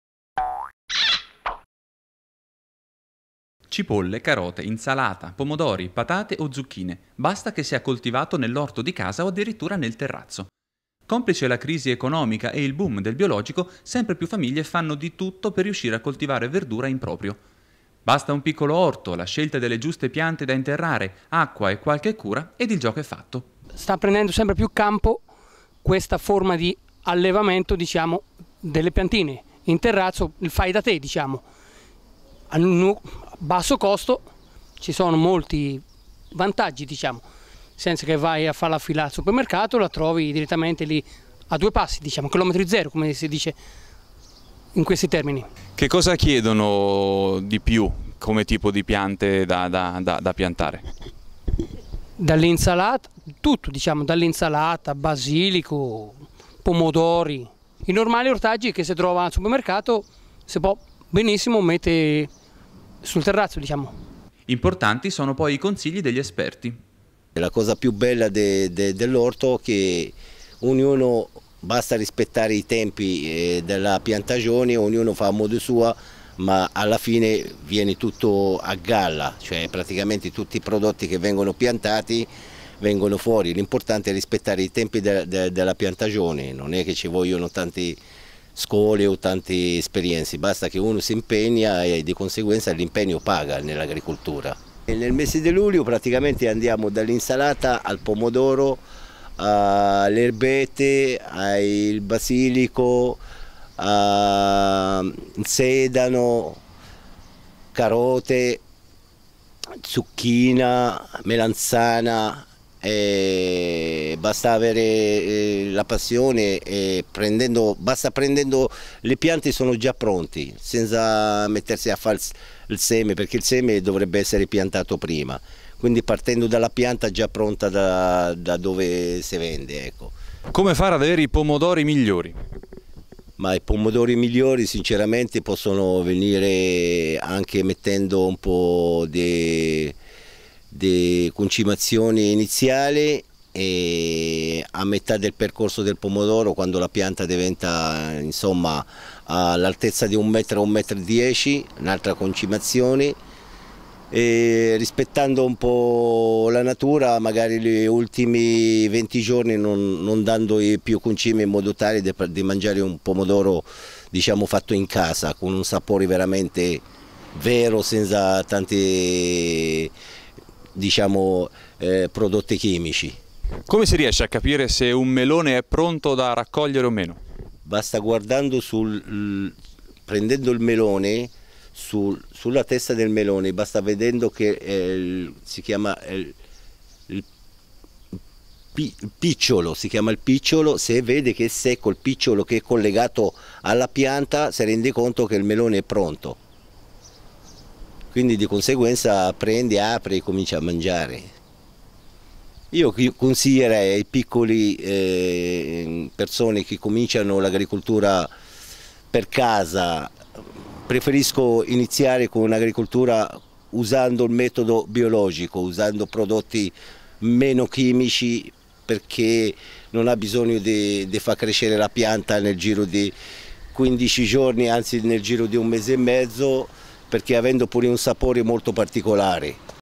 I segreti dell'orto "fai da te"
Cipolle, carote, insalata, pomodori, patate o zucchine, basta che sia coltivato nell'orto di casa o addirittura nel terrazzo. Complice la crisi economica e il boom del biologico, sempre più famiglie fanno di tutto per riuscire a coltivare verdura in proprio. Basta un piccolo orto, la scelta delle giuste piante da interrare, acqua e qualche cura, ed il gioco è fatto.